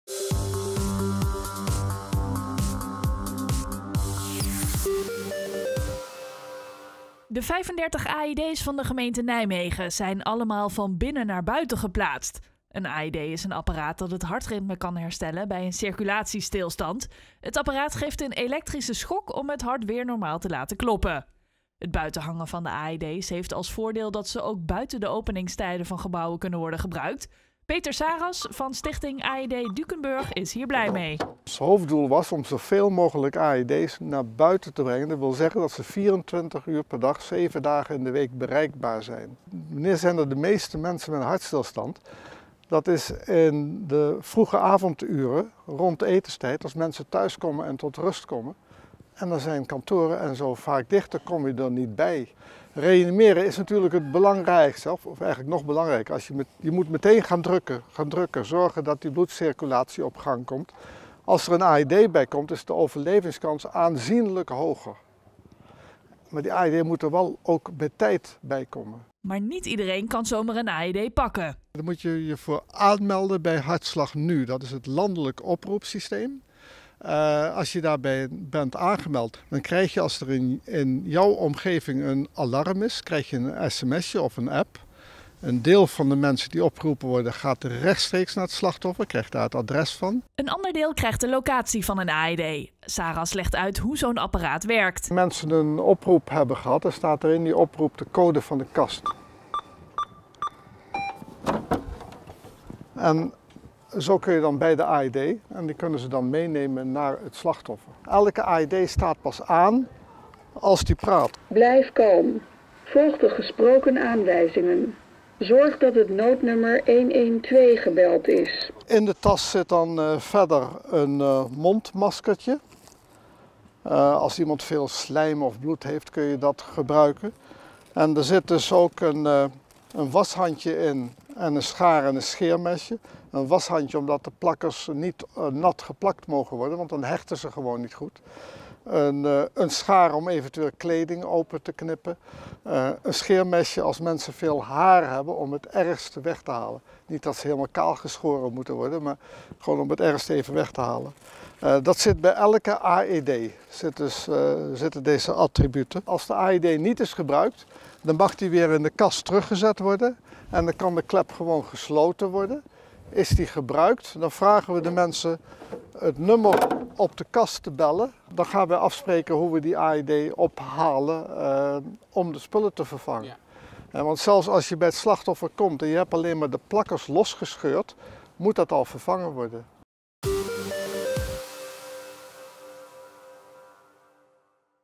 35 gemeentelijke AED's hangen nu buiten
De 35 AED’s van de gemeente Nijmegen zijn allemaal van binnen naar buiten geplaatst. Het initiatief kwam van de stichting ‘Hartslag Nijmegen.’ Het doel van de stichting is om overal in Nijmegen op zes minuten loopafstand een AED te hebben hangen. Het buiten hangen van de apparaten heeft volgens Hartslag Nijmegen als voordeel dat ze ook buiten de openingstijden van de gebouwen gebruikt kunnen worden.